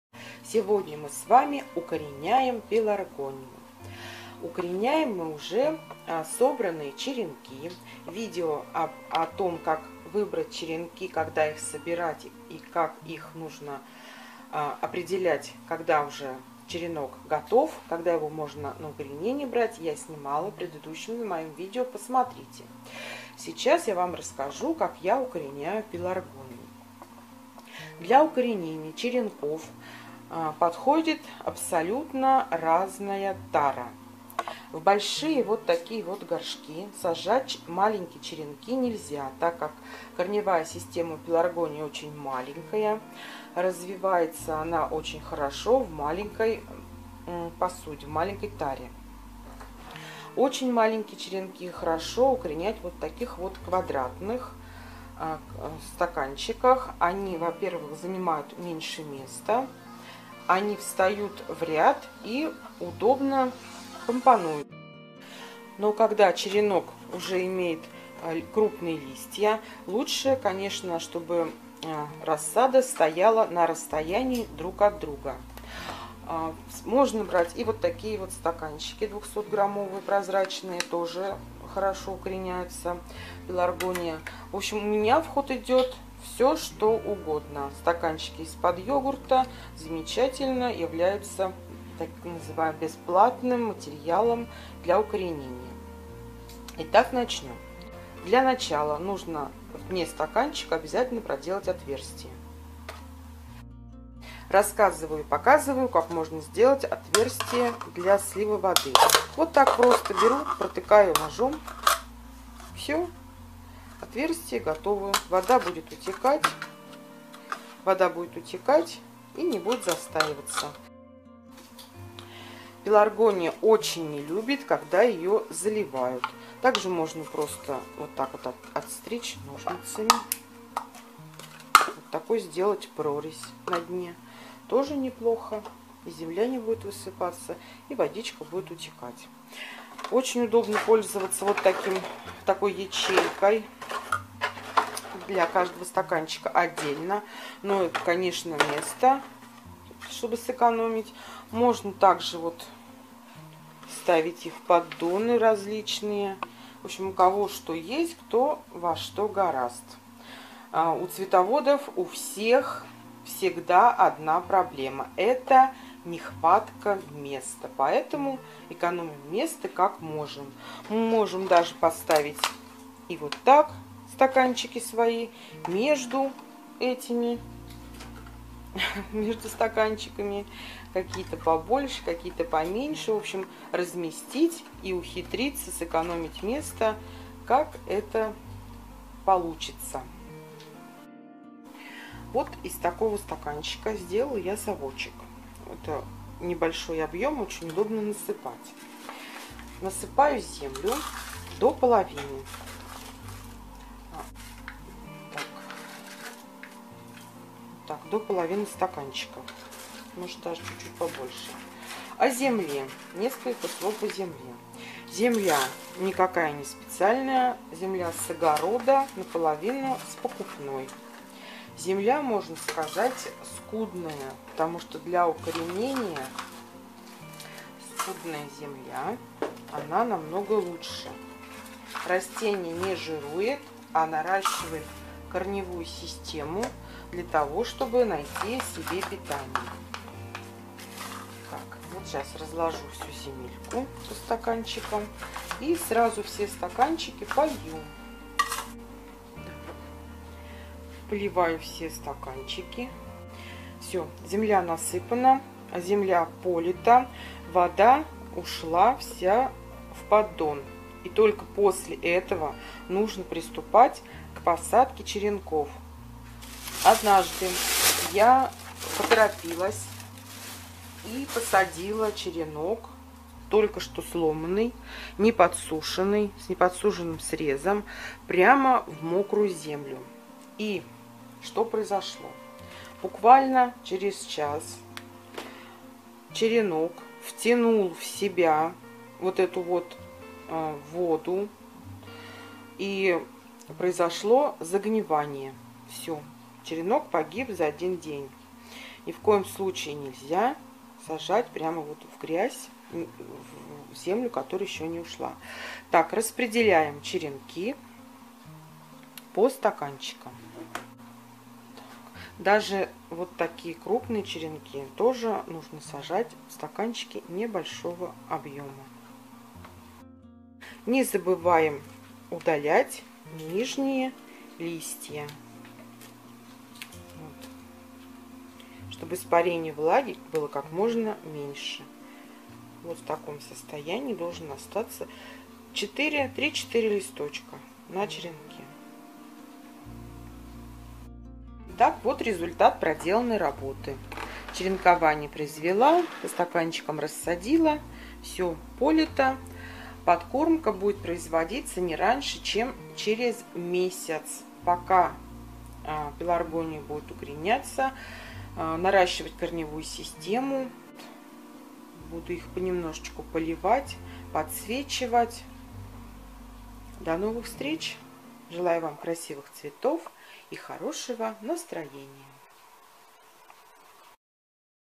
Посадка черенков пеларгонии
Наступил день посадки черенков пеларгонии. Для укоренения черенков я использую временные стаканчики из под йогуртов. Главное чтобы объём не превышал 100 мл. Землю беру скудную с огорода. Предварительно промораживаю, добавляю немного покупной земли. Черенки необходимо дополнительно досвечивать, умеренно поливать. Первая подкормка через месяц комплексным удобрением для комнатных цветов.